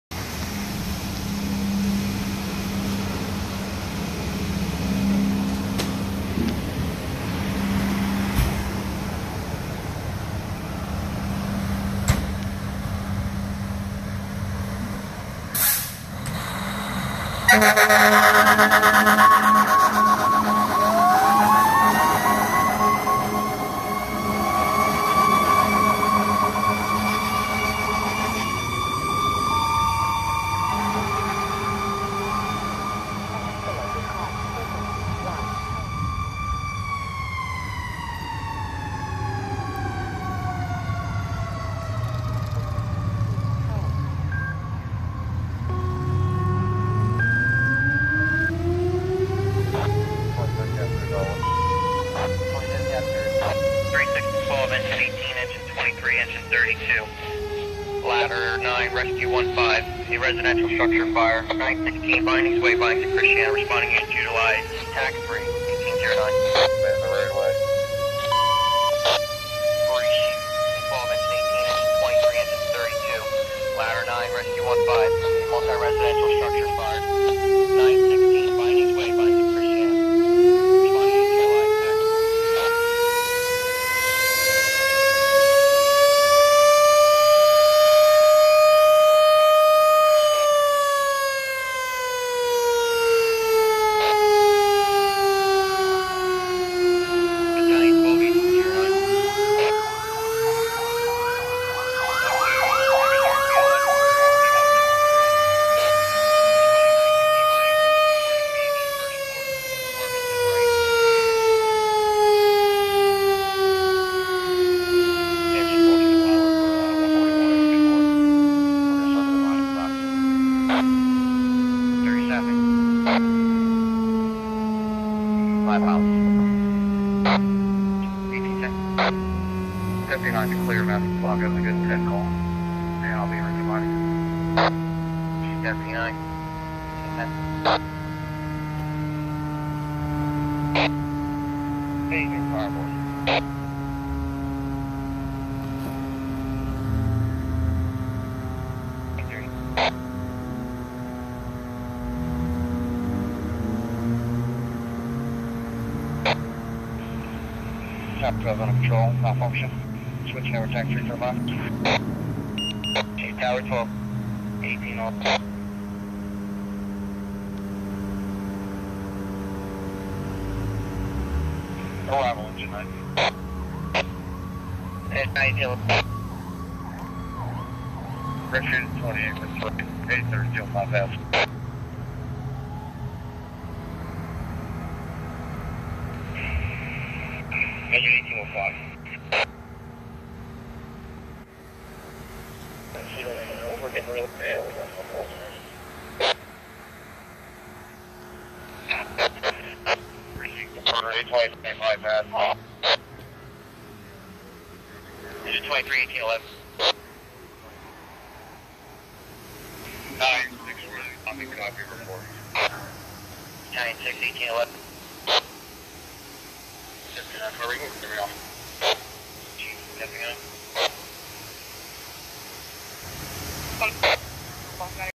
Newport, Delaware E234 ,out to assist Co 12 on a building fire
Old Baltimore Pike area